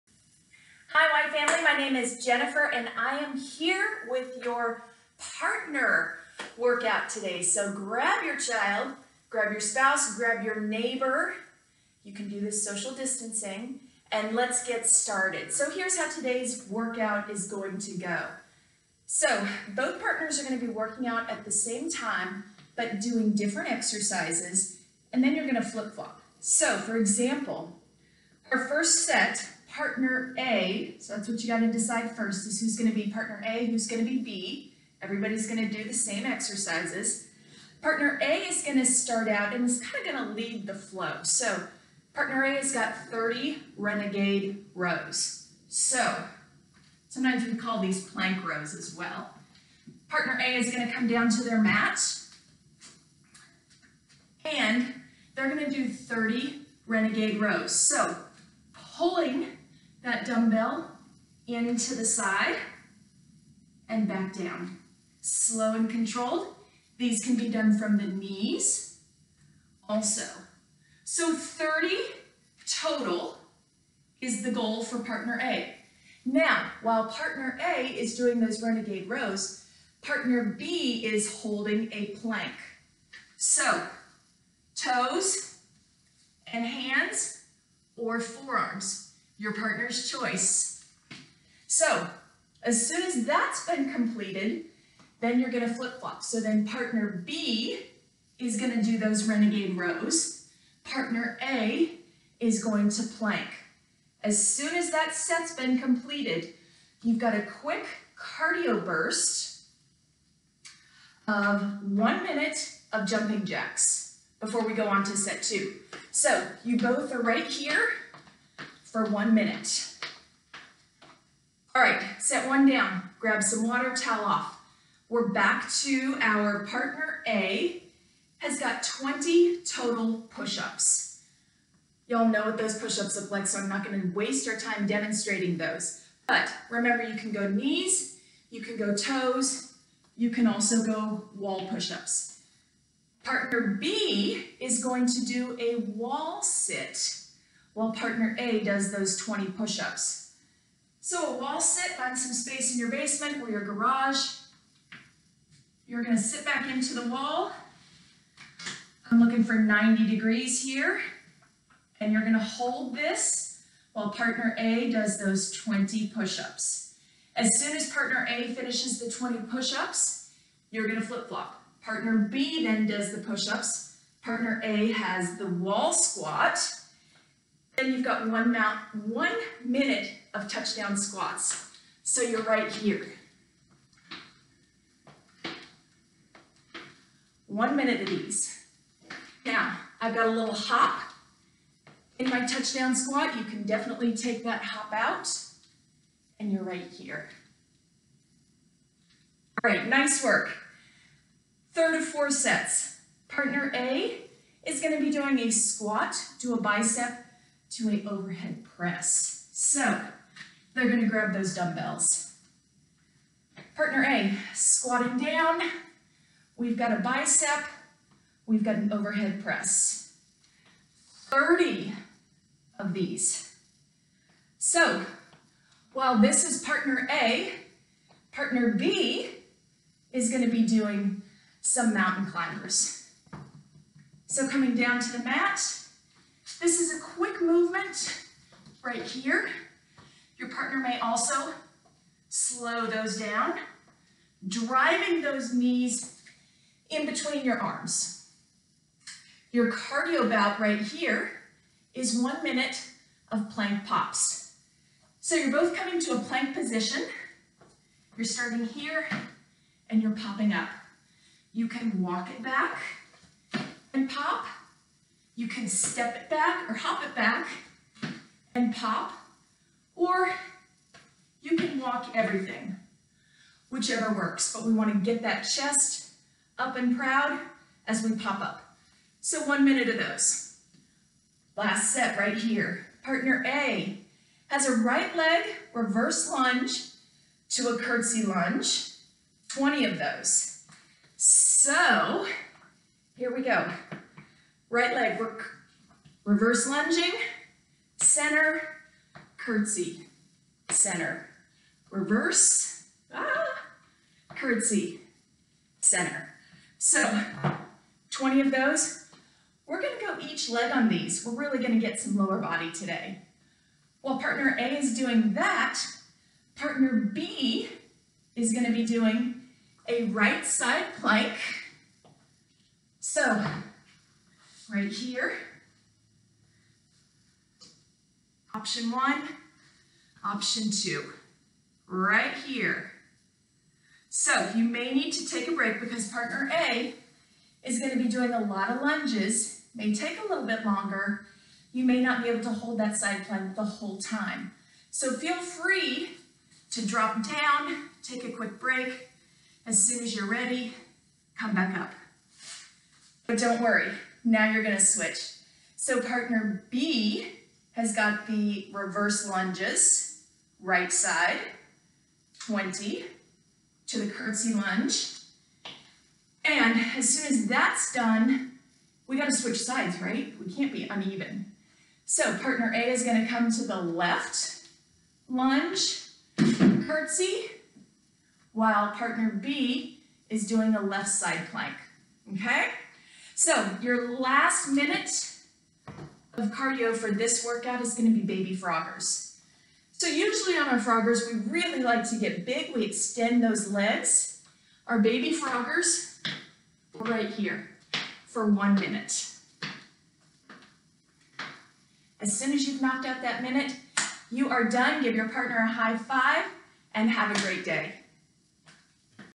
Workout of the Day - Partner Workout
Partner Workout - this is a great workout to do with your child or significant other!  Get everyone moving together.  First, decide who is partner A and who is partner B. Then get to work!

Partner A - Renegade Rows - 30 Total
Partner B - Plank Hold until partner is done
Switch - A becomes B and B becomes A
Both Partners - 1 Minute of Jumping Jacks

Partner A - Pushups - 20 Total
Partner B - Wall Squat until partner is done
Both Partners - 1 Minute of Touchdown Squats

Partner A - Squat to Bicep and Overhead Press - 30 Total
Partner B - Mountain Climbers until partner is done
Both Partners - 1 Minute of Planks Pops

Partner A - Right Leg Reverse Lunge to Curtsy Lunge - 20 Total
Partner B - Right Side Plank until partner completes 10 reps and then switch sides
Both Partners - 1 Minute of Baby Froggers

Now high five your partner! You’re all done!